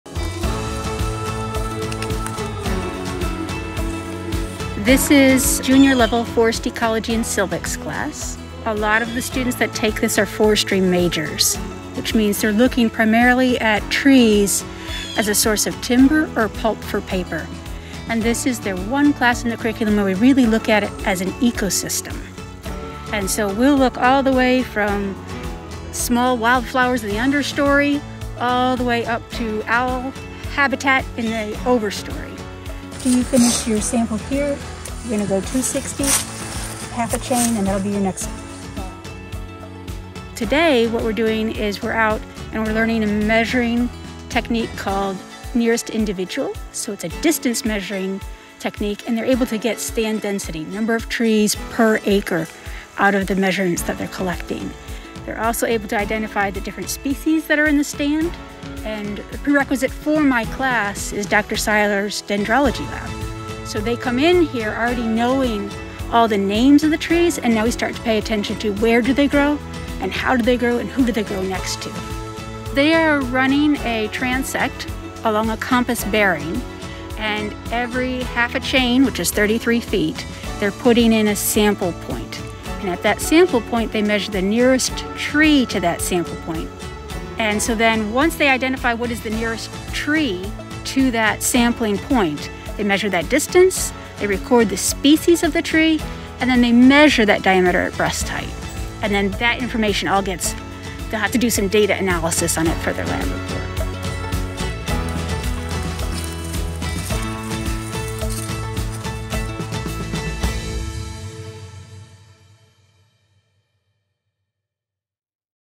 Forestry students learn through hands-on experience
Students in Carolyn Copenheaver's Forest Ecology and Silvics class are learning a measuring technique that allows them to determine the number of trees per acre. Additionally, students learn about the environmental factors affecting the establishment, growth, and development of forests; silvical characteristics of trees; forest community structure and function; and forest ecosystem analysis.